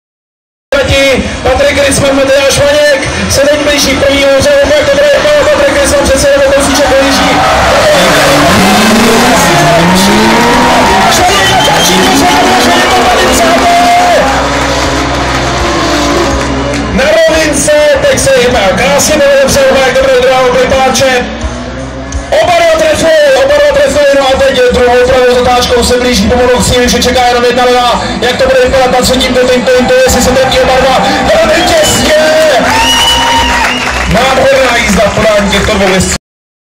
CDS Sosnová 09/12 Ritzmann vs. Vaněk
Jeden z battlů mezi BMW E30 s kompresorem přeplňovaným motorem a BMW E46 s motorem 2JZ-GTE s výkonem 573 koní